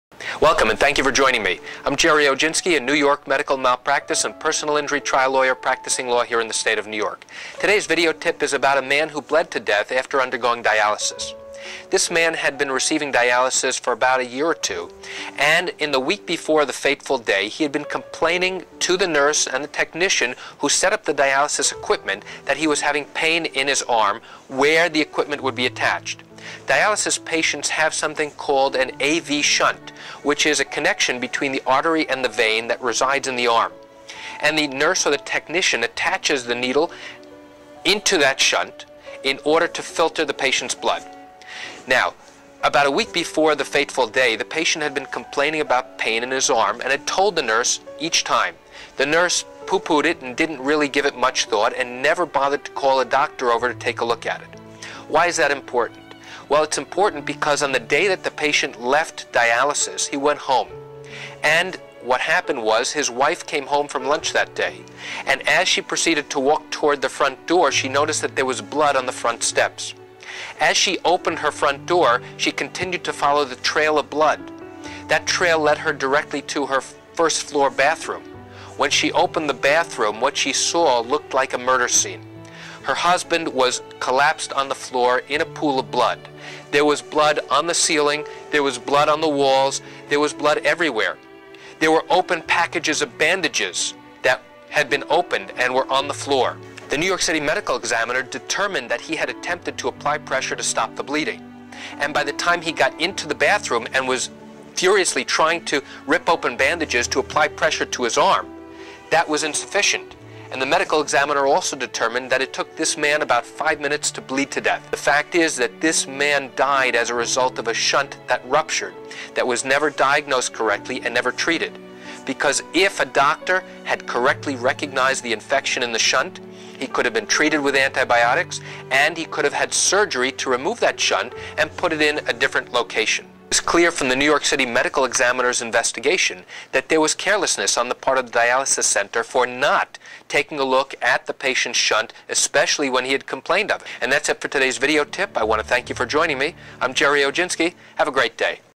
New York Medical Malpractice-Death From Ruptured Shunt
Listen in as Gerry Oginski, an experienced New York medical malpractice, negligence, wrongful death and personal injury trial lawyer practicing law in Manhattan, Queens, Brooklyn, Bronx, Staten Island, Nassau & Suffolk explains how a failure to diagnose an infection in a dialysis patient caused him to bleed to death.

The Law Office of Gerald Oginski, LLC